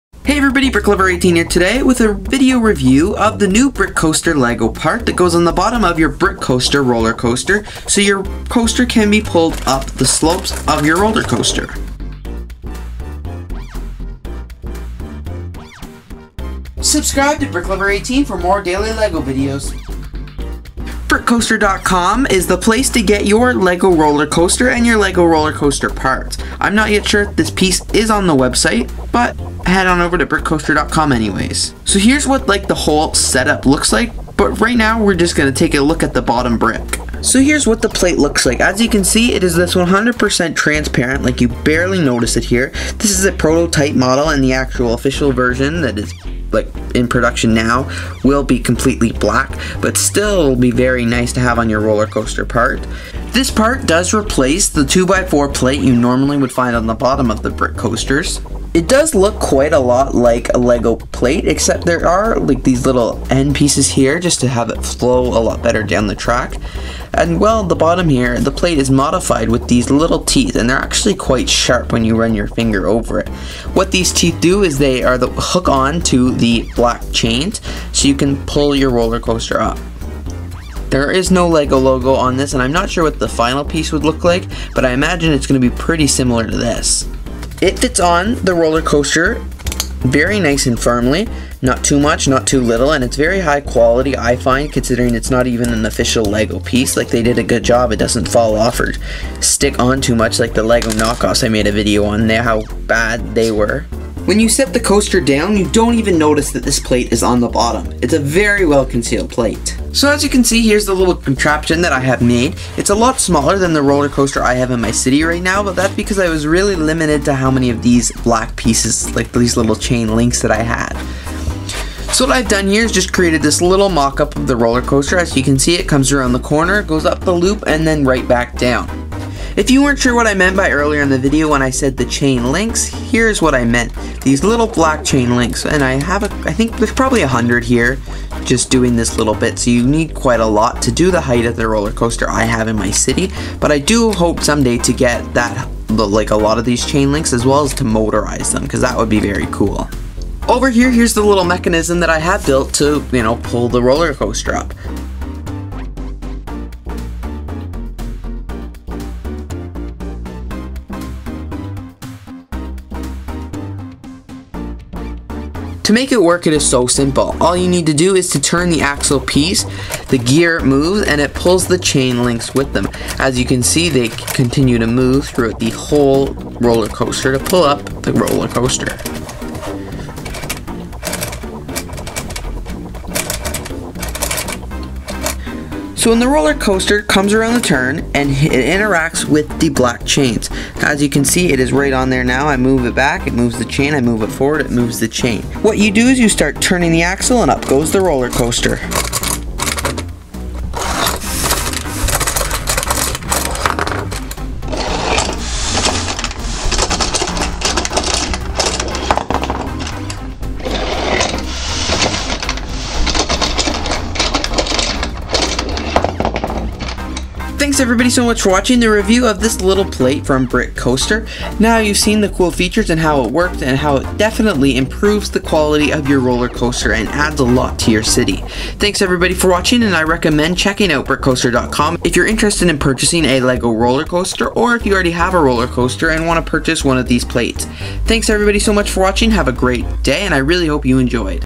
LEGO ROLLER COASTER LIFT REVIEW AND HOW TO (FROM BRICK COASTER)
This new plate from Brick Coaster changes the entire feel and functionality of my LEGO Roller Coaster!

(this video was an unpaid product placement)